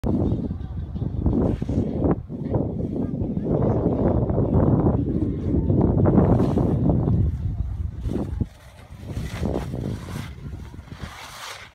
ピーコック
坂だから回るのか、坂だから回りにくいのか？？？
ま、いいからどんどこ回ってみよう‼️

Ski Dance Association of Japan
日本スキーダンス協会